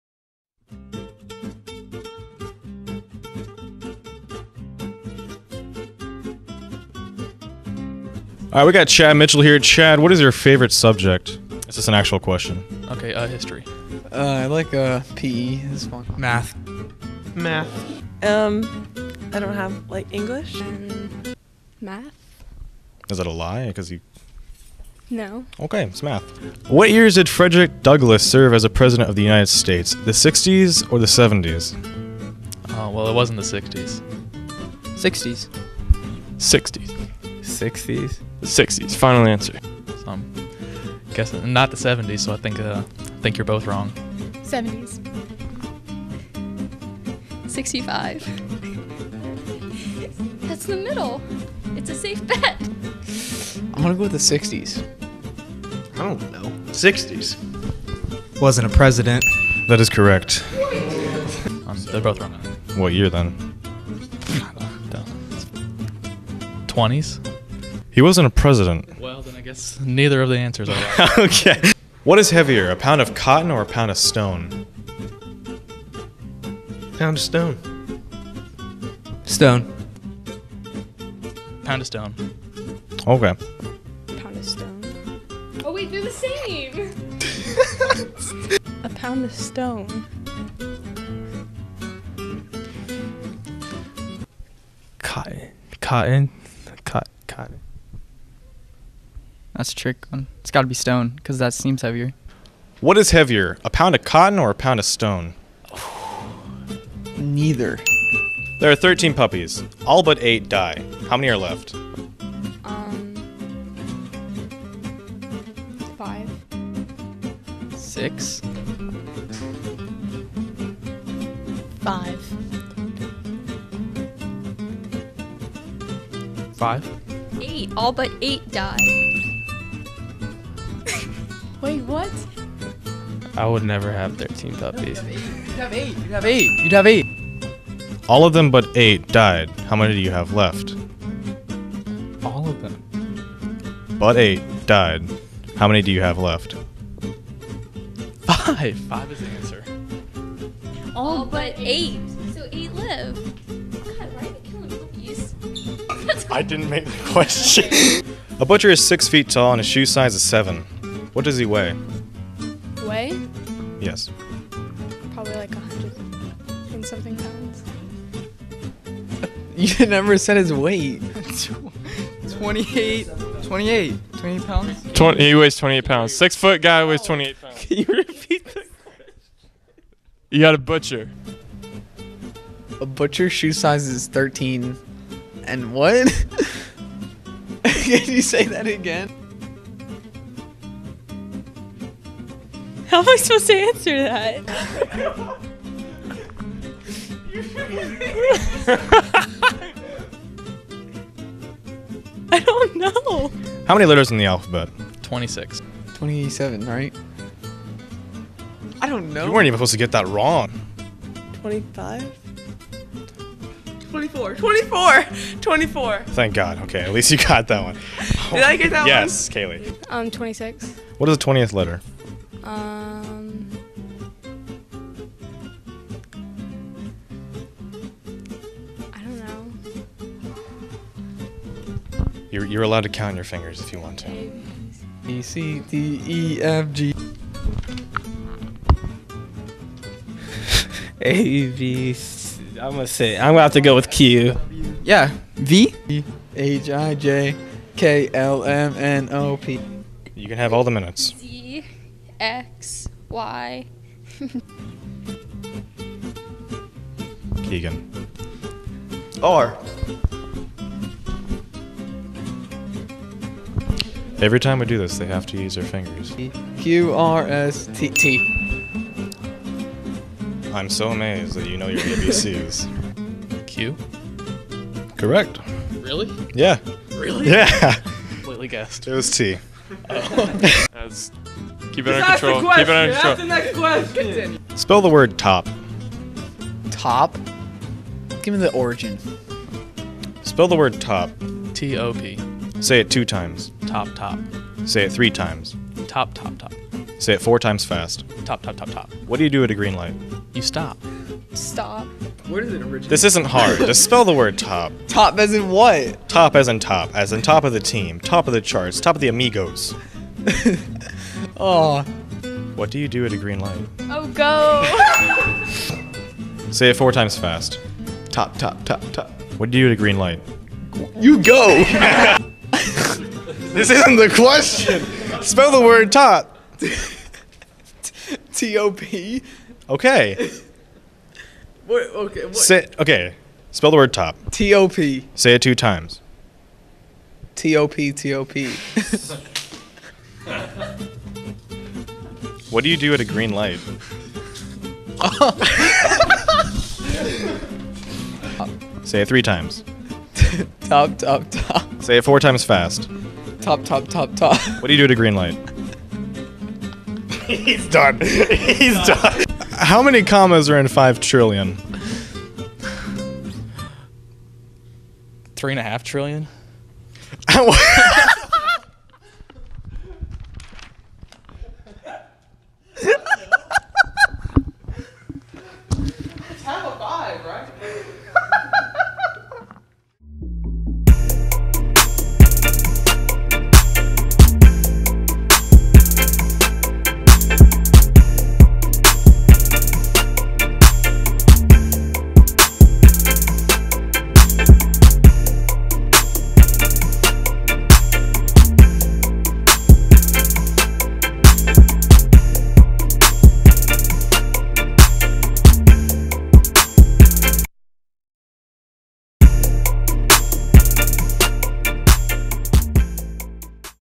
Bishop Questions
Bishop McLaughlin Catholic High School students Alec Tharin and Carson Ragsdale ask other students questions and get surprising results.